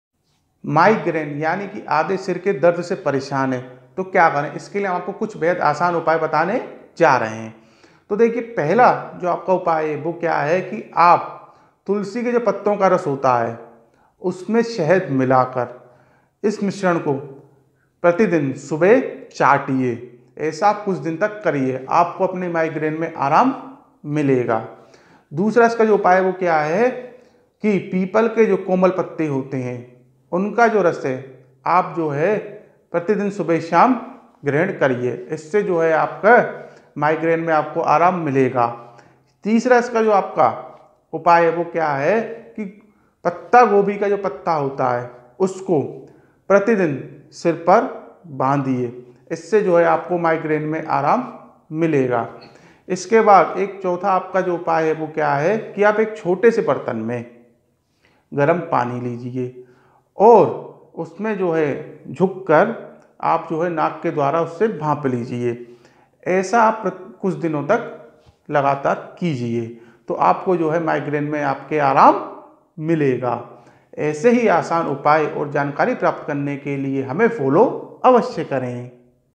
Migraine treatment, Migraine ka ilaj, माइग्रेन का घरेलू उपचार, आधे सिर में दर्द के आयुर्वेदिक नुस्खे
इस वीडियो में माइग्रेन के विषय में चर्चा की गयी है,

video highlights:-